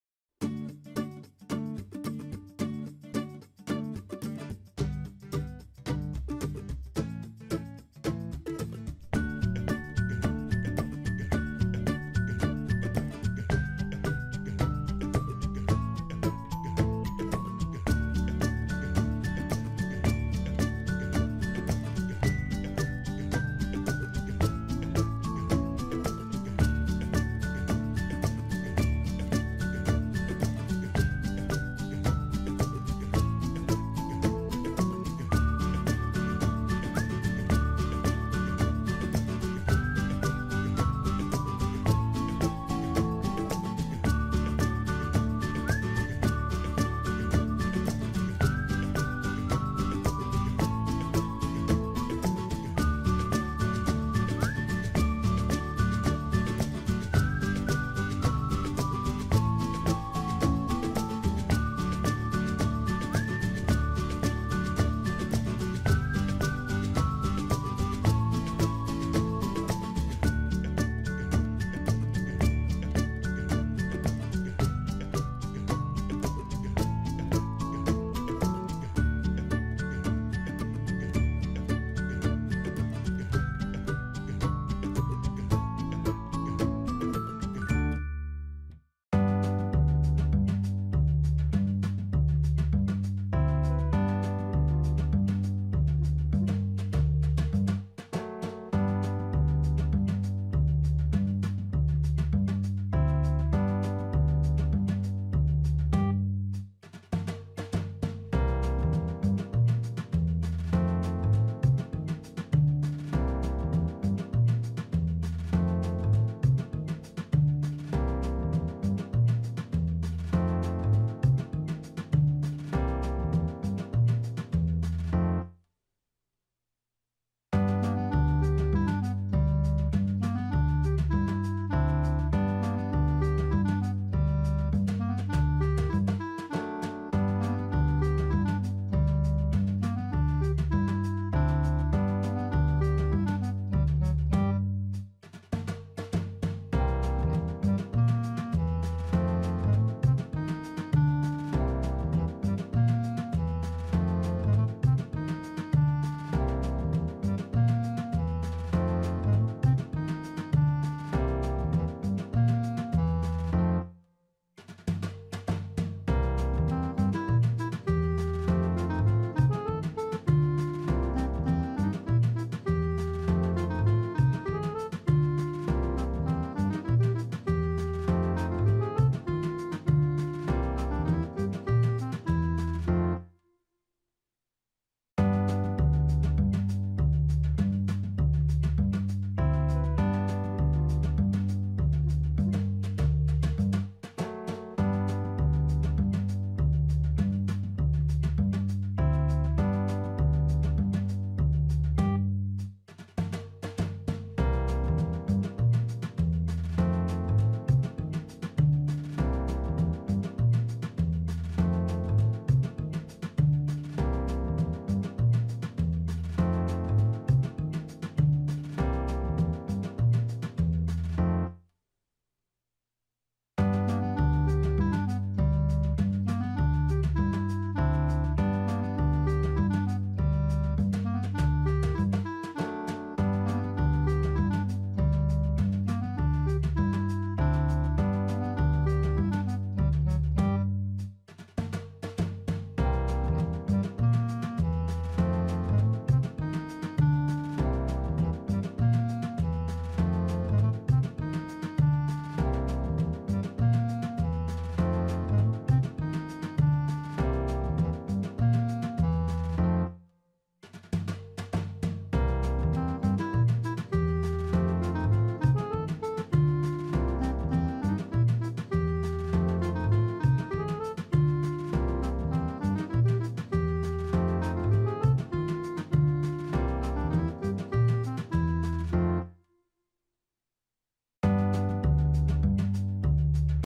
Wie man Kämpfer aus Papier | Papierflieger selbst basteln 🔥
Schwierigkeit: einfach
Papier größe : a4

Es war ein sehr schnell fliegendes Papierflieger. sehr einfach zu machen. Sie können dieses Flugzeug falten, indem Sie die Schritte im Video befolgen. Mach es jetzt ruhig :)